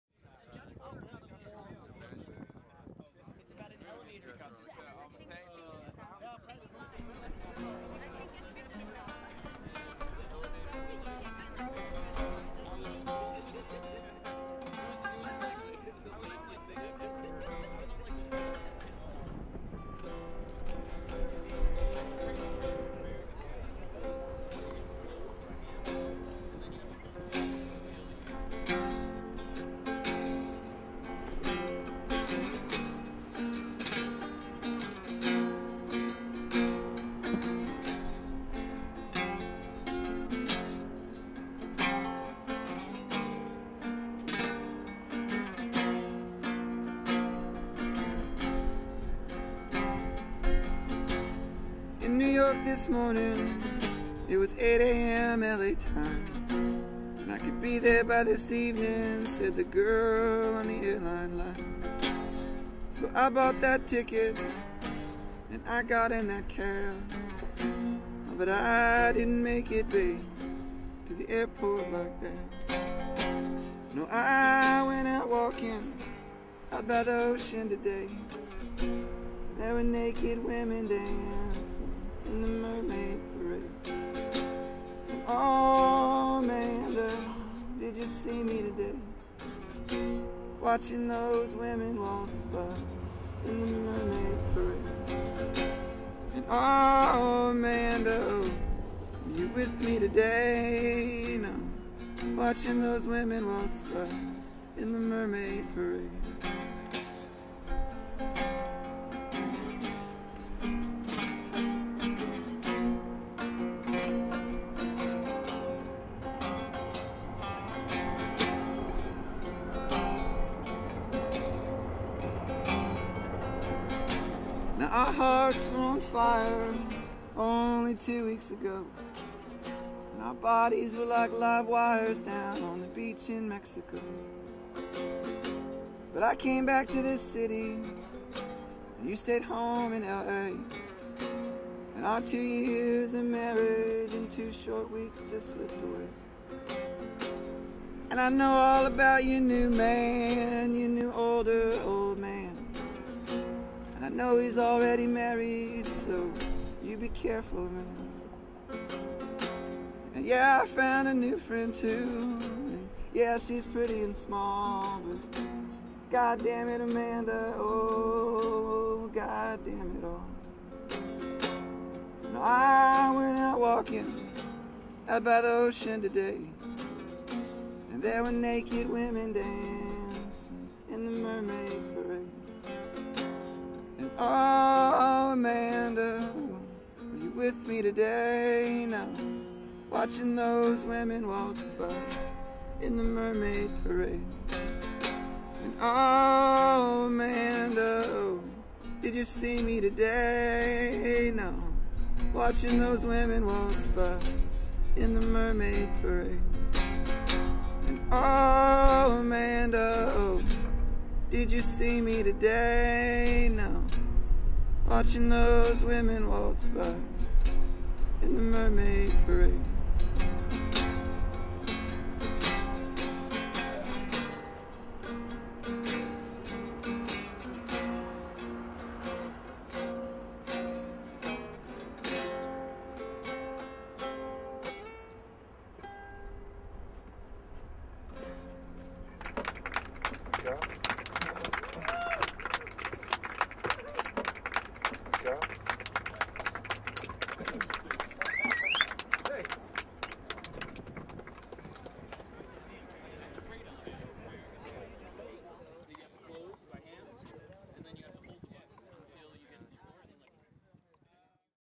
rooftopsession phosphorescent Subbacultcha
Phosphorescent had a spheric show on the rooftop of the Subbacultcha building. The views are spectacular, the cocktails tasted great and the melacholic music made you feel that you're driving in a old mustang cabrio in the desserts of the USA. Great moment.